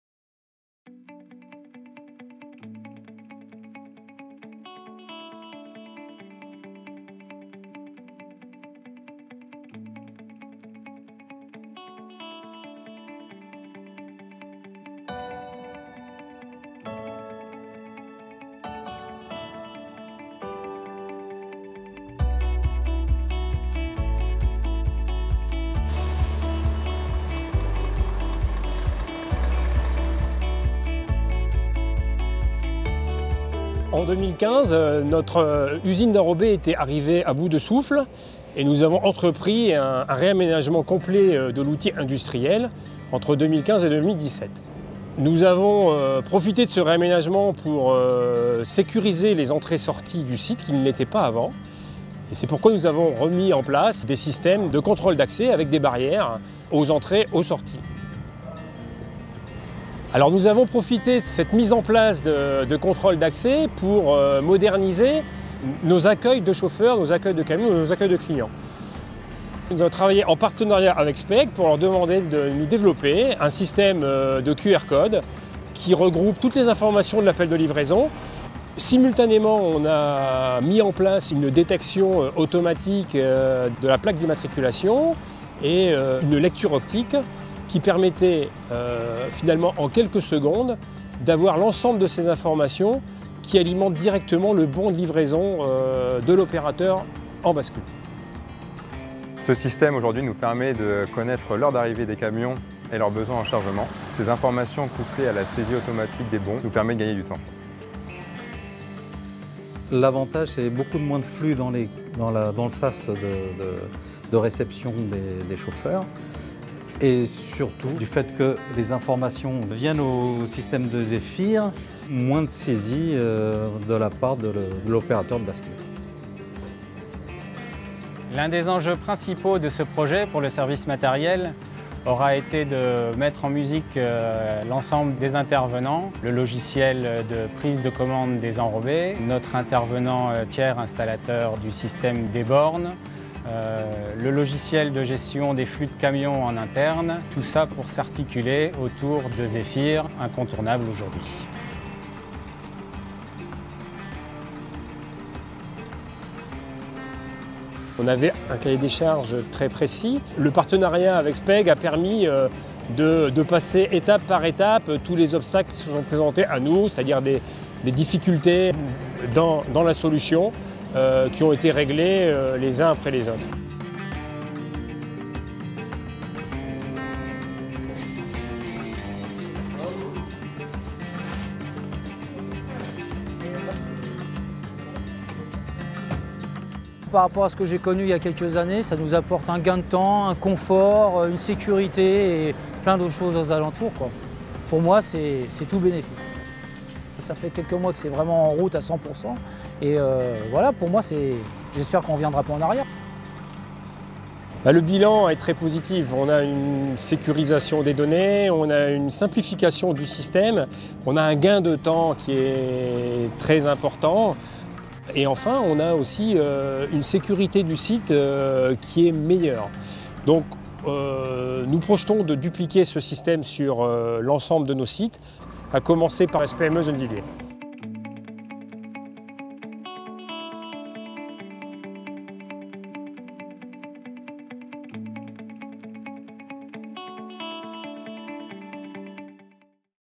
Le poste d'enrobé 2.0 !
La SPME, filiale de Colas Île-de-France Normandie, a réalisé le réaménagement complet de son site de Bonneuil-sur-Marne (94) en partenariat avec la filiale informatique Speig. Entrées et sorties mieux sécurisées, accueil des chauffeurs modernisé, logistique améliorée, gains de temps... Le nouveau dispositif offre de nombreuses avancées et sera déployé à l’ensemble des sites de la SPME.